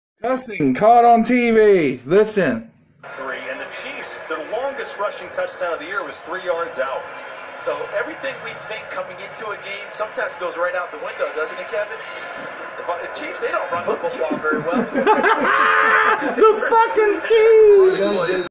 Announcer Drops F-Bomb on Live TV
Many people believe Charles Davis dropped the F-Bomb in this video. What do you guys hear? Comment your thoughts.👇🏼 Footage: The East Sider